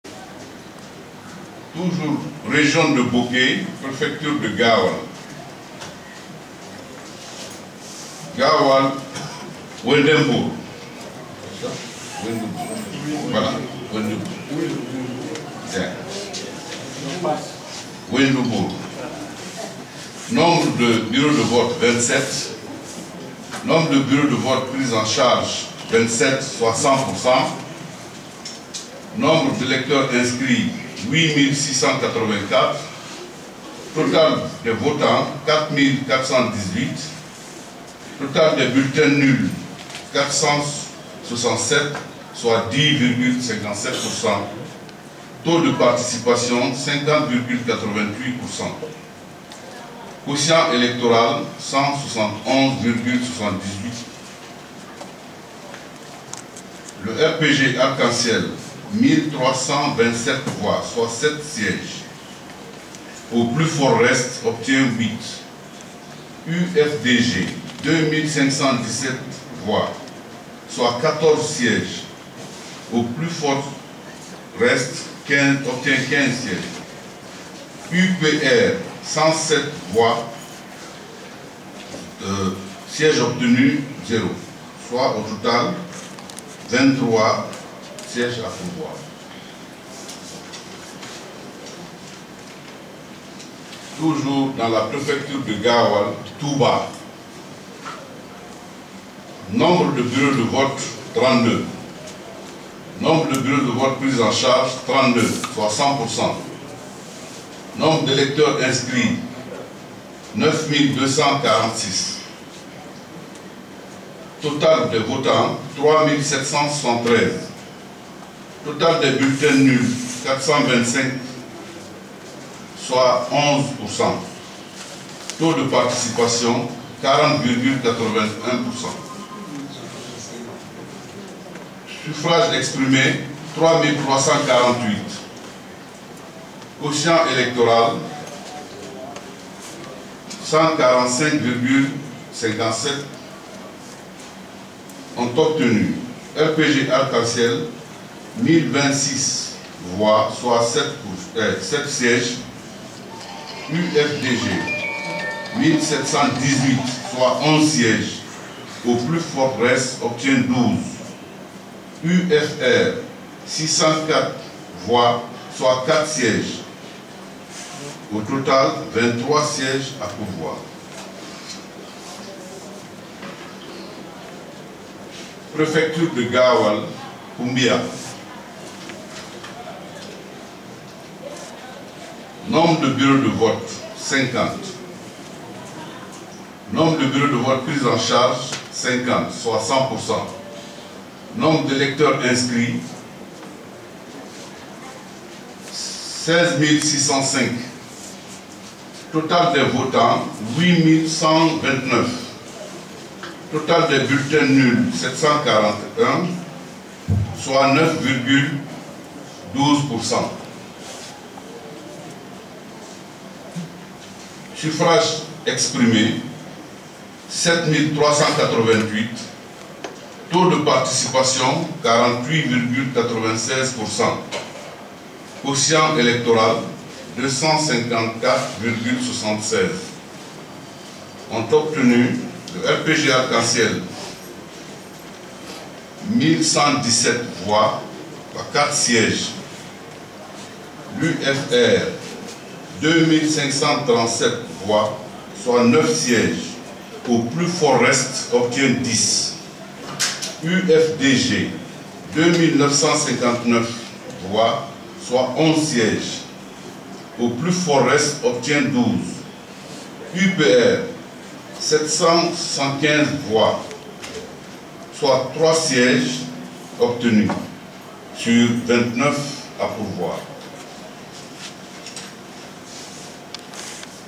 Résultats des CR de Wendou Bour, Touba et de Koumbia (préfecture de Gaoual)
Résultats des CR de Wendou Bour,  Touba et de Koumbia (préfecture de Gaoual) de l'élection communale du 04 février 2018